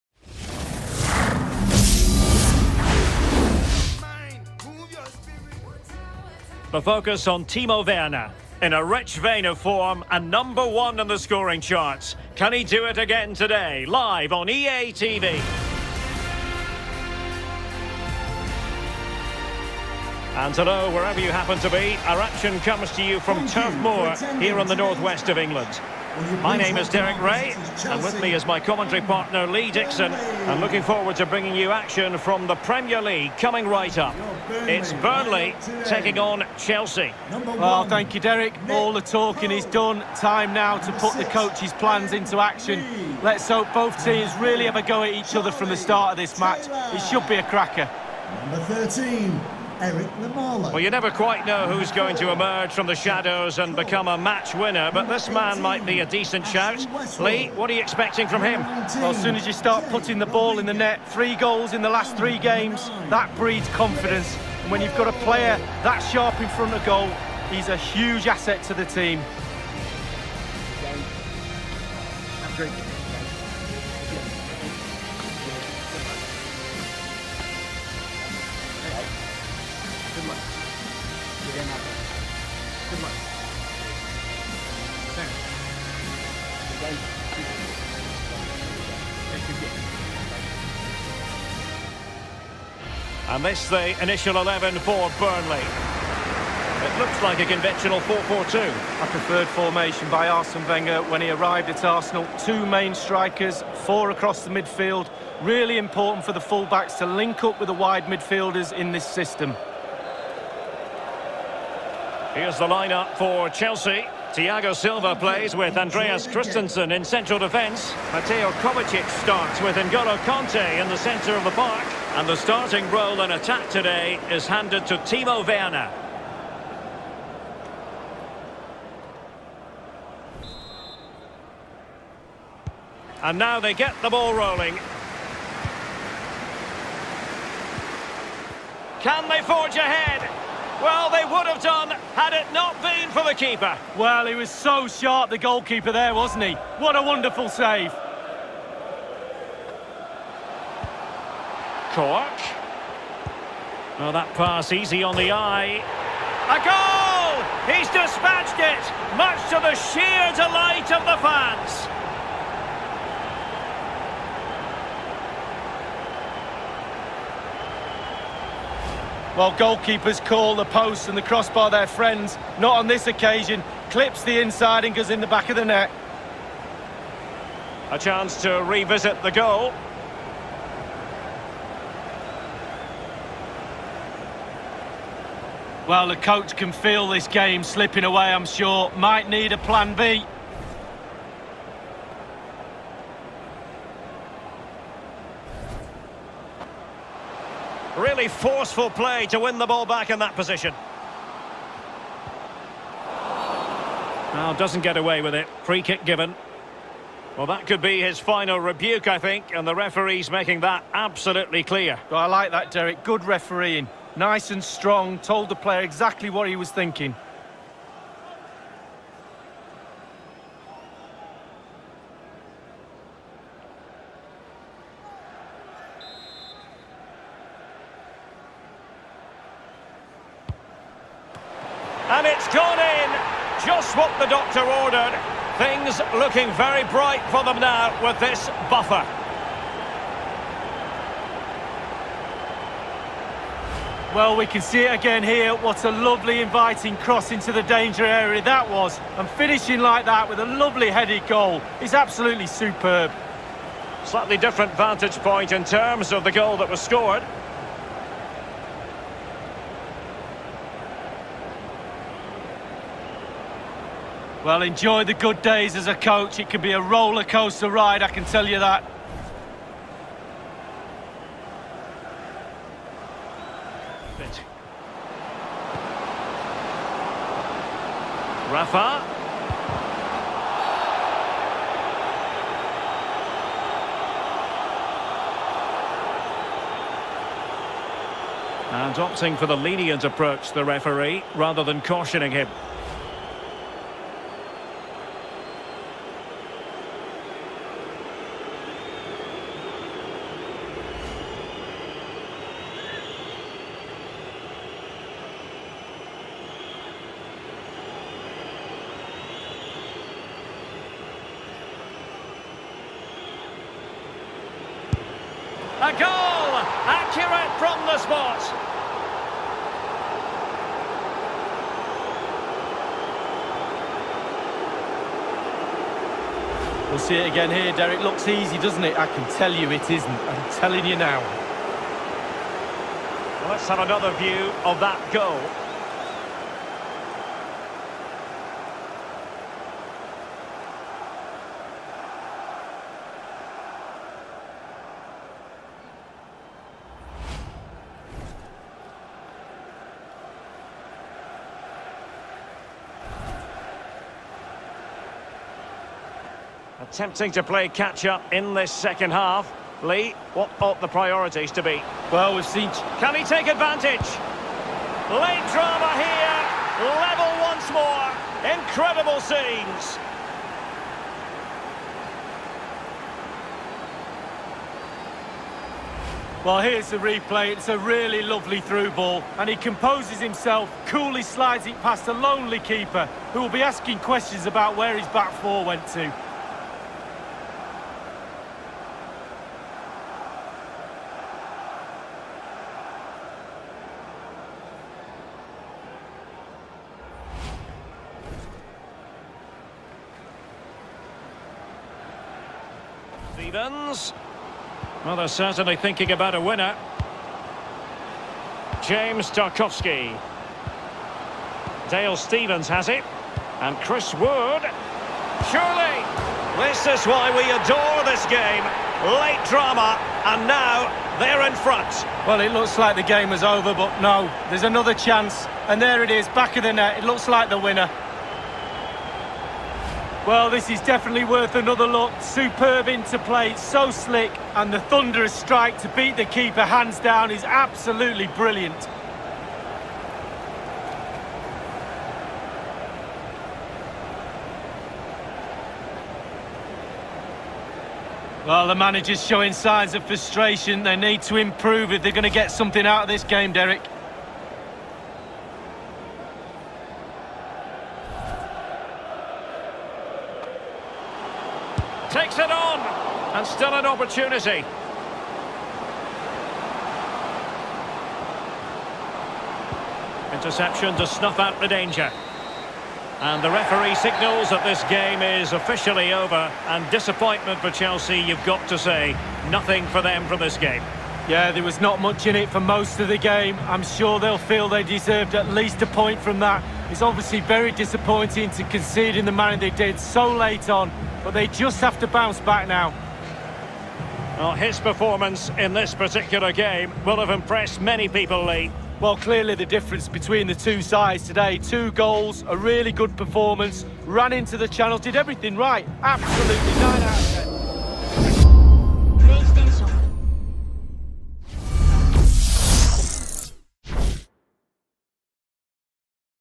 Burnley Vs Chelsea - Premier League highlights
Premier League highlights of Chelsea Vs Burnley - Rafa makes his debut for the Blues